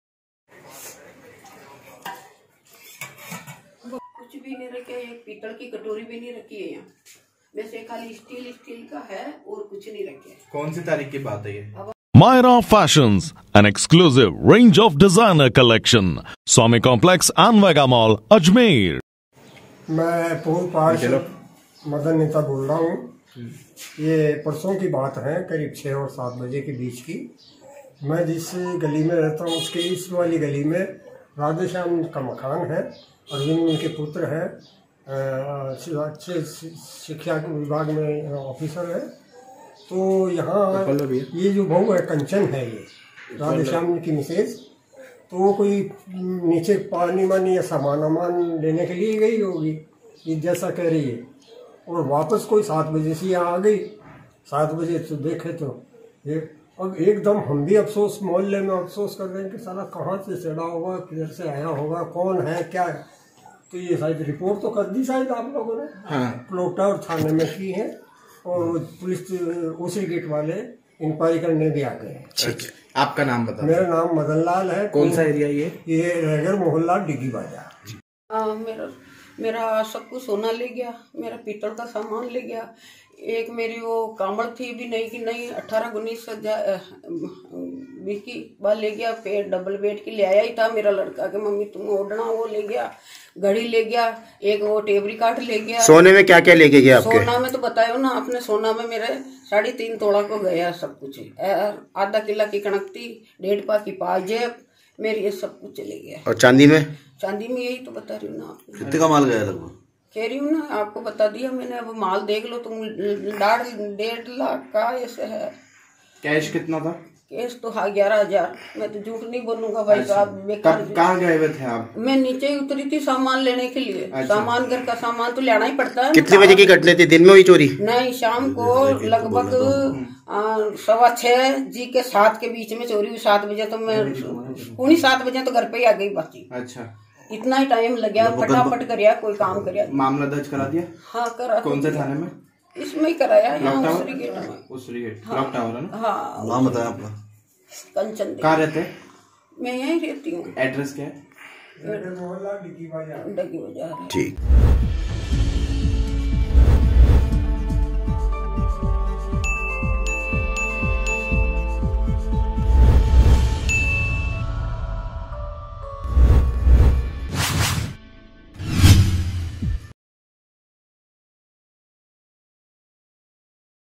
AJMER NEWS | क्लॉक टावर थाना इलाक़े के रेगर मोहल्ला डिग्गी चौक में हुई चोरी | MTTV INDIA
क्लॉक टावर थाना इलाक़े के रेगर मोहल्ला डिग्गी चौक में हुई चोरी,मकान मालिक सामान लेने गया था बाजार,चोरों ने करीब 2 लाख का माल किया पार पीड़ित ने क्लॉक टावर थाने में दी चोरी की शिकायत